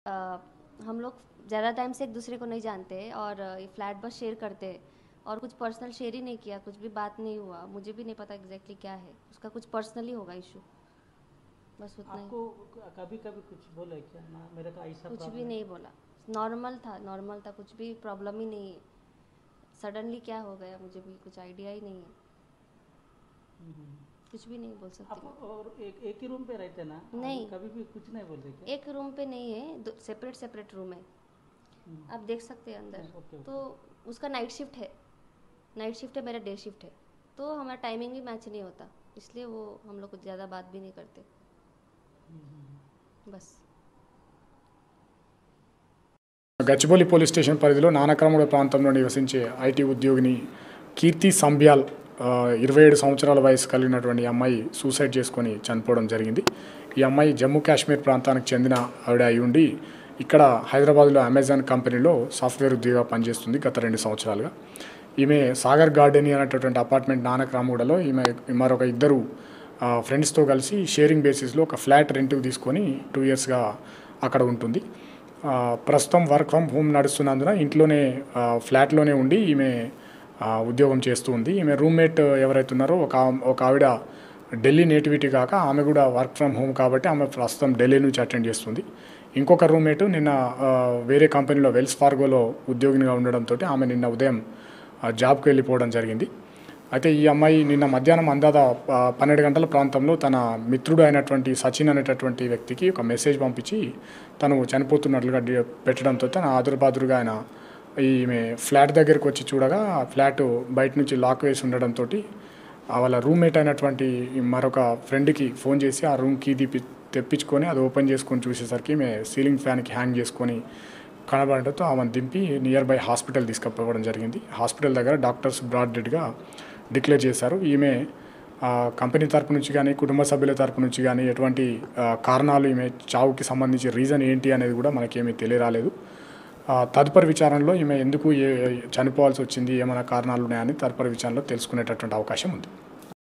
GACHIBOWLI POLICE LIMITS AMAZON COMPANY MEIN JOB KARNE WALE 27sala KIRTHI NA APNI FLAT MAI DI J**N.
JBTv News#GACHIBOWLI POLICE LIMITS AMAZON COMPANY MEIN JOB KARNE WALE 27.sala KIRTHI NA APNI FLAT MAI DI J**N...! JO KASHMIR KI RAHANE WALI THI..